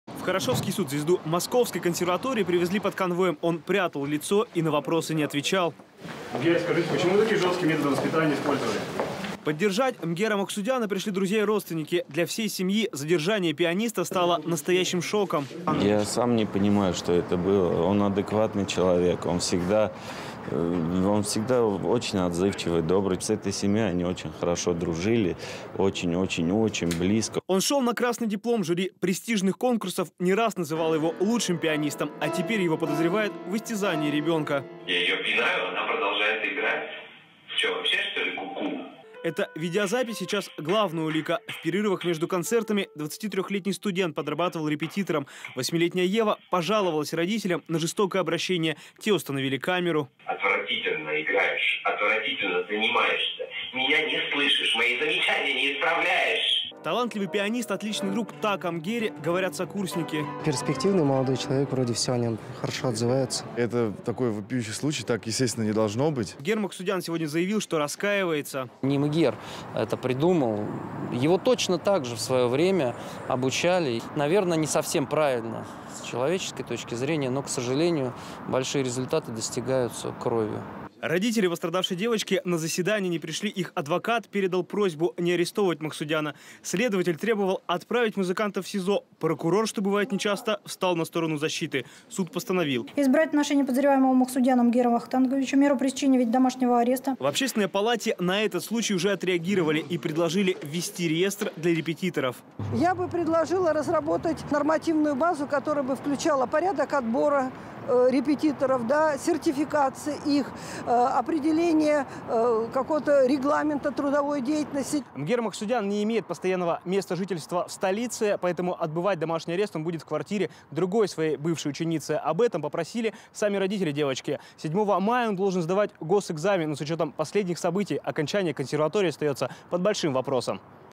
Пианиста Махсудяна поместили под домашний арест
Пианиста Махсудяна поместили под домашний арест 26 04 19